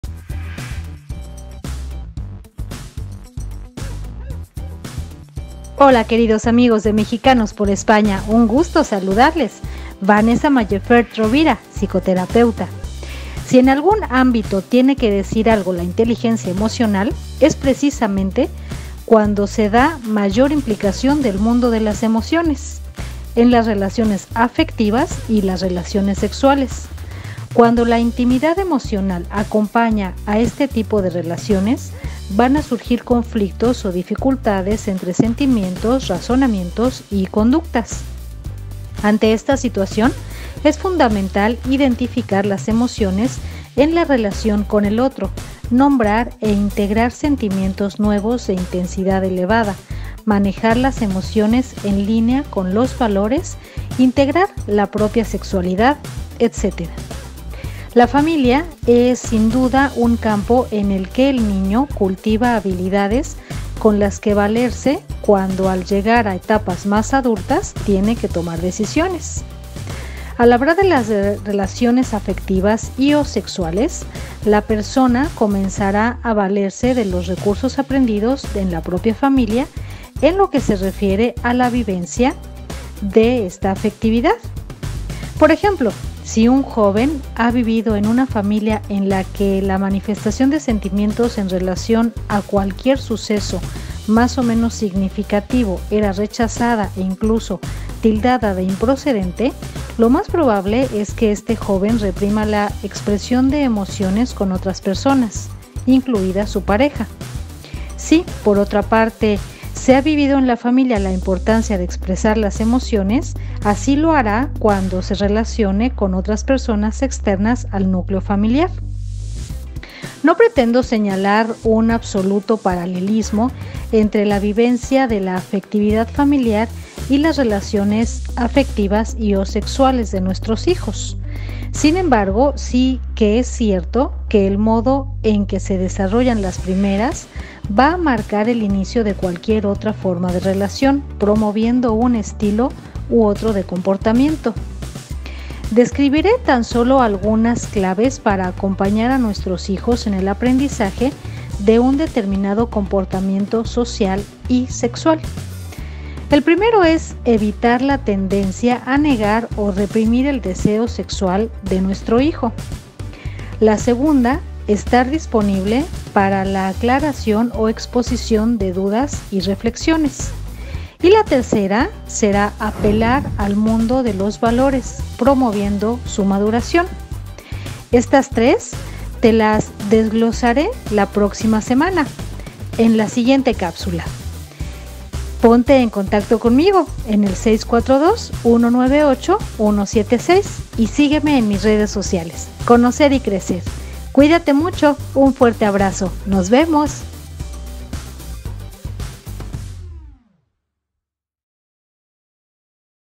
E236T5 💢 Vanessa Maillefert Rovira define:💬 “Sexualidad y relaciones afectivas”
🎤 «Conocer y crecer»
💢 Vanessa Maillefert Rovira define:
💬 “Sexualidad y relaciones afectivas”

👁‍🗨 Psicoterapeuta dedicada a la  atención, intervención, acompañamiento y seguimiento de personas, parejas y familias, brindando recursos, estrategias y herramientas orientados a la solución de problemas desde su sección dominical.

♥️ Nuestra misión:
¡Ayudar a la comunidad mexicana 🇲🇽 impulsando su desarrollo!